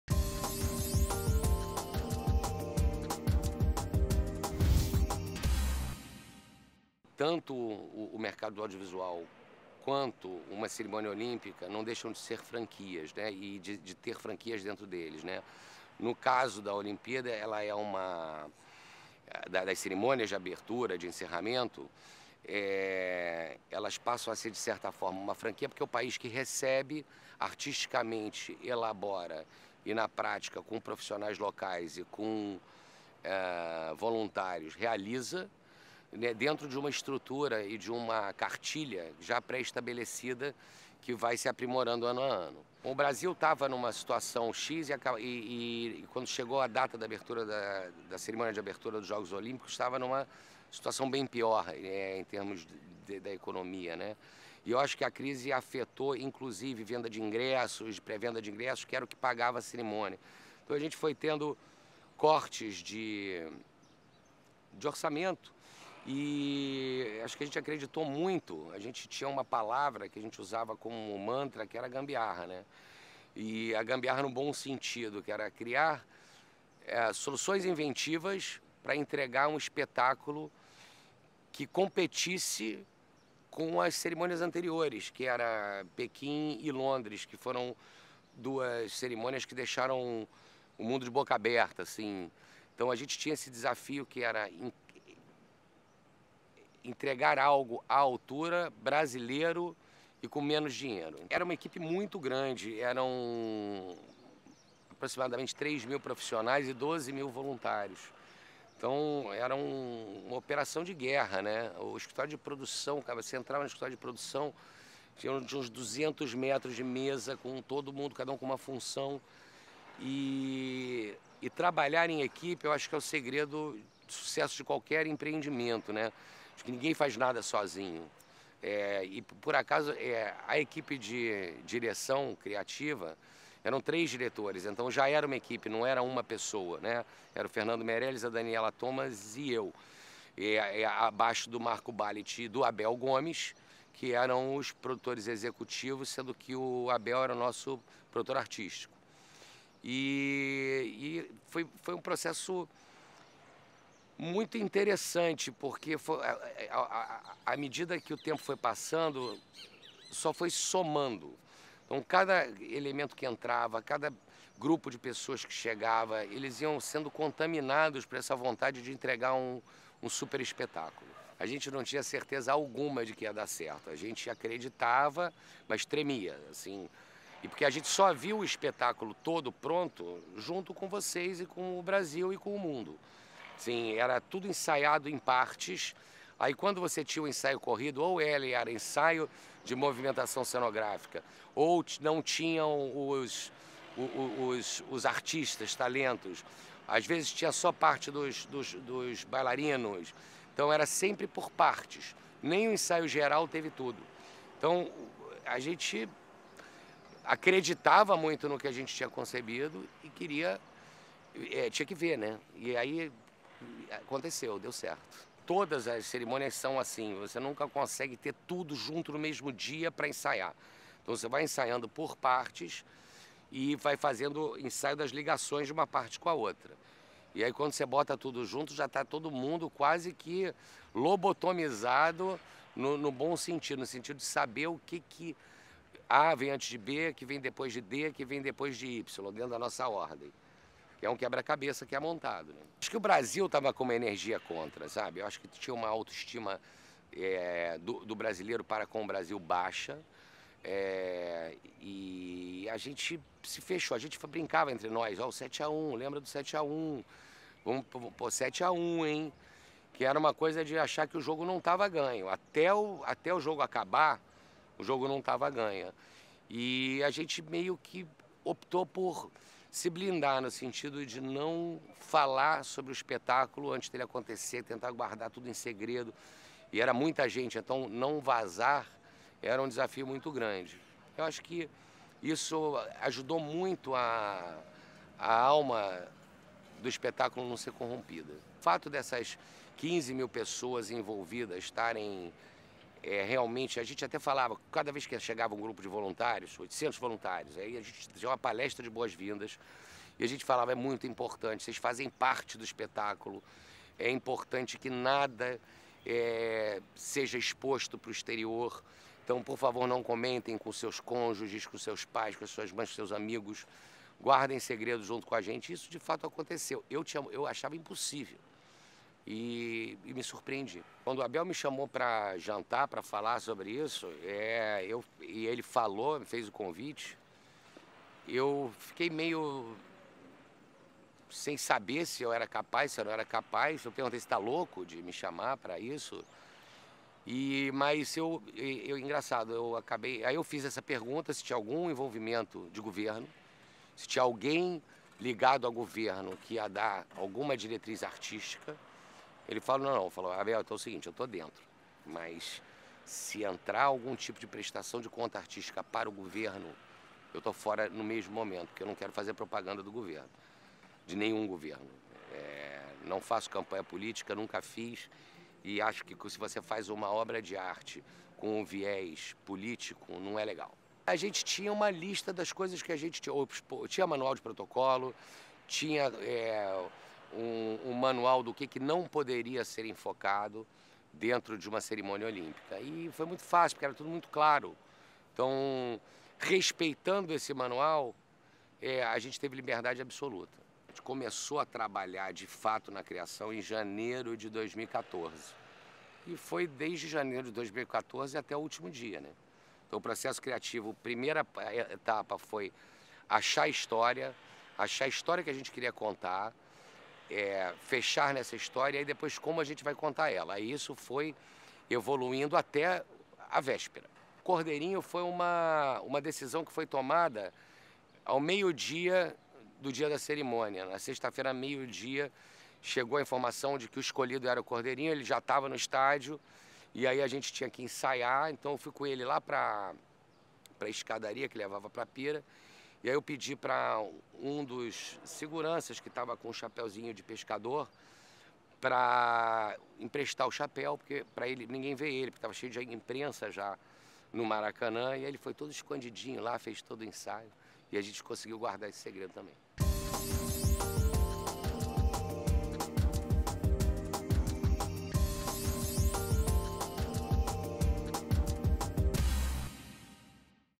Andrucha Waddington - 16ª Convenção ABF do Franchising
Andrucha Waddington comenta a relação do mercado audiovisual com as franquias. Fala também sobre à realização da cerimônia de abertura das Olimpíadas, os processos pré estabelecidos que devem ser atendidos pelos países sede e como trabalharam para encontrar soluções para a realização da cerimônia no Brasil apesar da crise financeira.